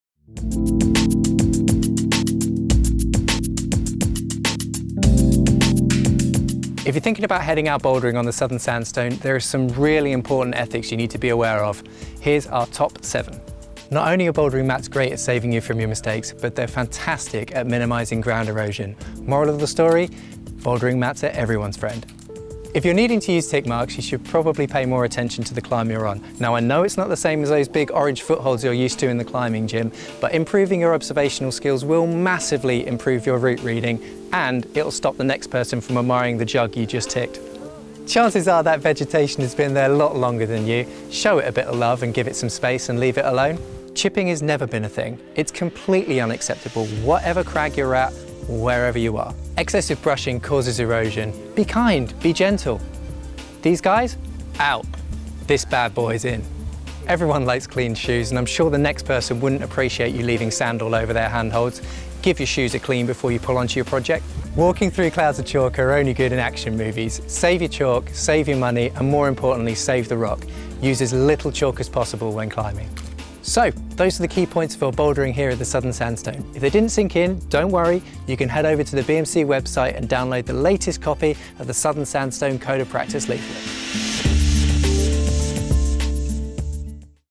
Southern Sandstone Bouldering: Respect the Rock
Respect the rock. Watch this and learn how to help protect the unique Southern Sandstone crags when you're bouldering.

How to boulder on Southern Sandstone:

1. Use bouldering pads to reduce ground erosion
2. Don't place tick marks
3. Don't remove turf or vegetation
4. No chipping!
5. Don't use a toothbrush or boulder brush
6. Clean your boots
7. Use as little chalk as possible

That's how to Respect the Rock